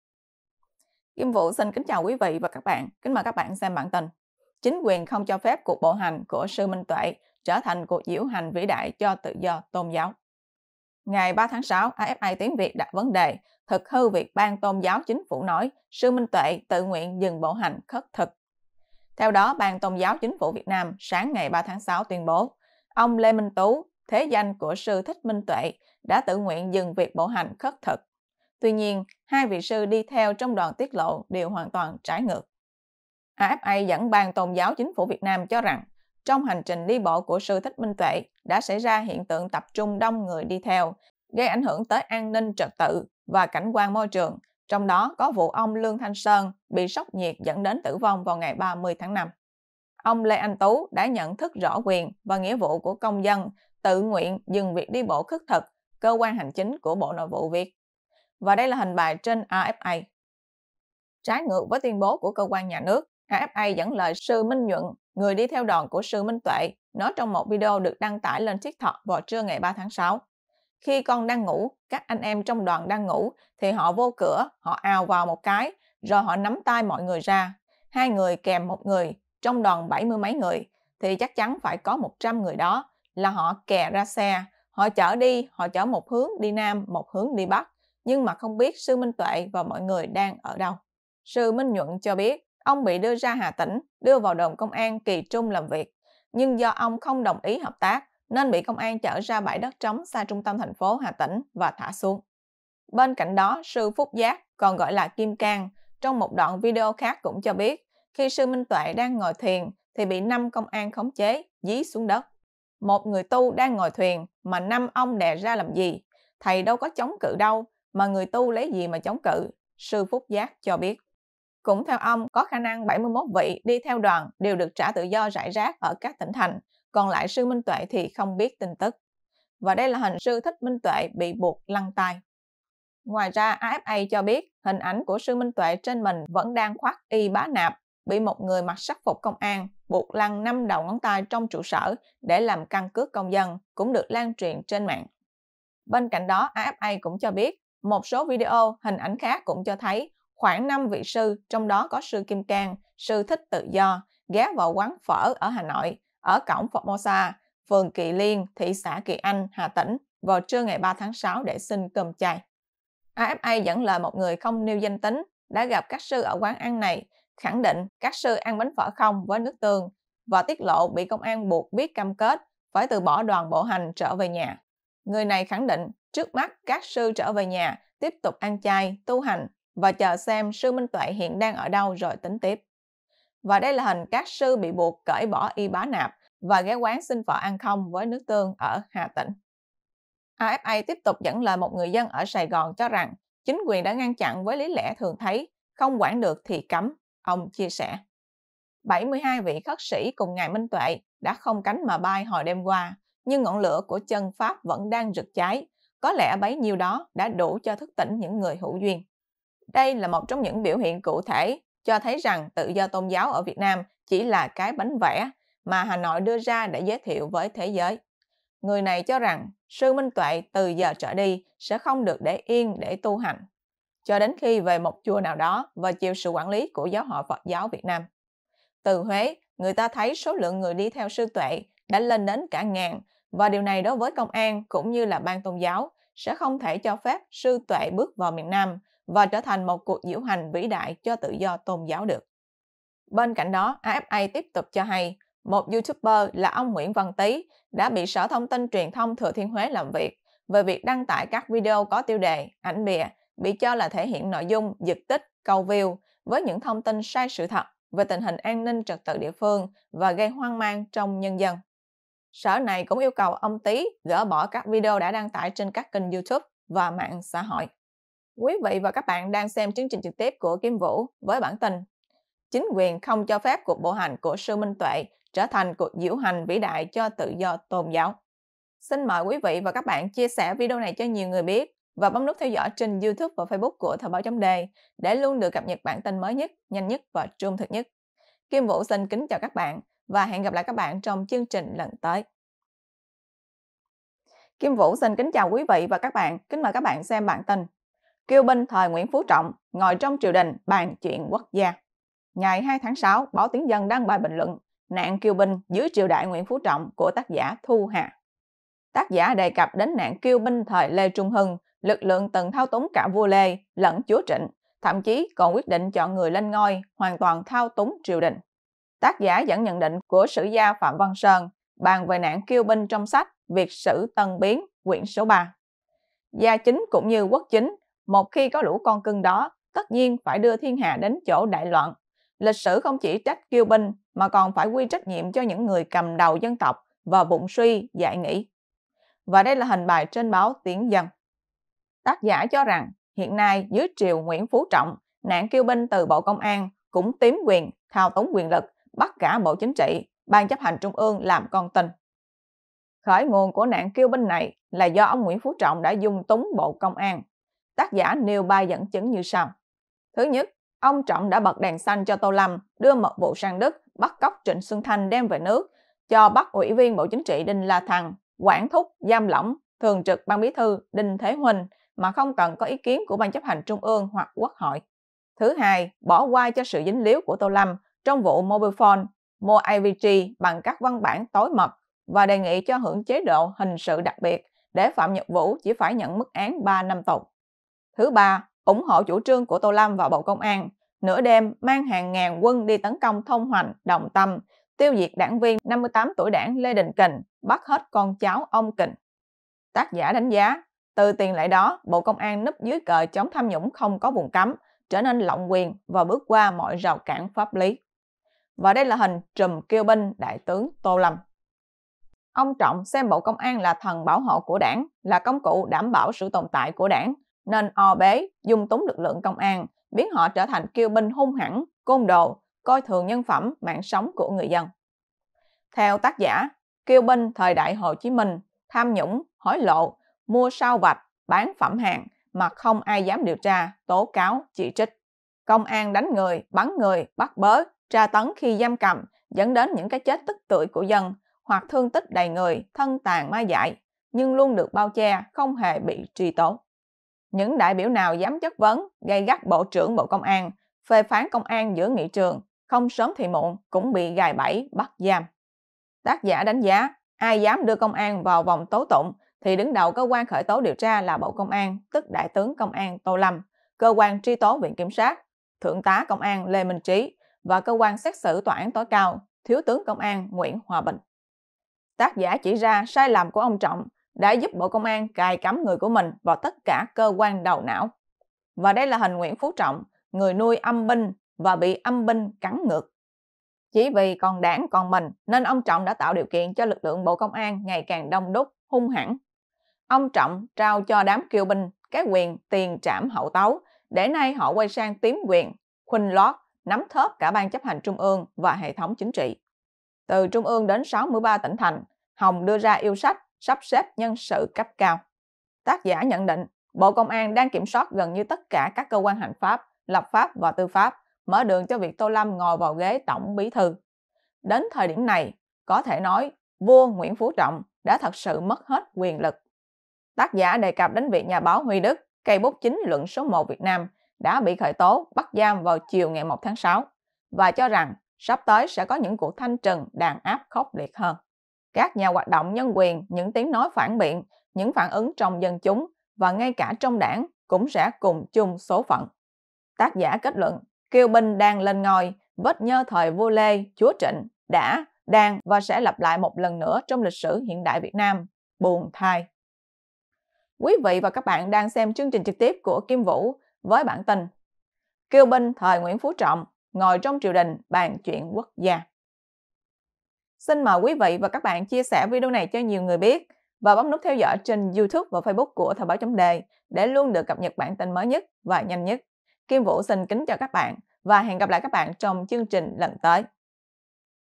Chính quyền lo sư Minh Tuệ trở thành cuộc diễu hành vĩ đại cho tự do tôn giáo
Ban Tôn giáo Chính phủ Việt Nam sáng 3/6 tuyên bố, ông Lê Anh Tú (thế danh của sư Thích Minh Tuệ) đã tự nguyện dừng việc bộ hành khất thực. Tuy nhiên, 2 vị sư đi theo trong đoàn tiết lộ điều hoàn toàn trái ngược. Dư luận cho rằng, công an cũng như là Ban Tôn giáo, không thể cho phép sư Minh Tuệ bước vào miền Nam, và hành trình của thầy trở thành một cuộc diễu hành vĩ đại cho tự do tôn giáo.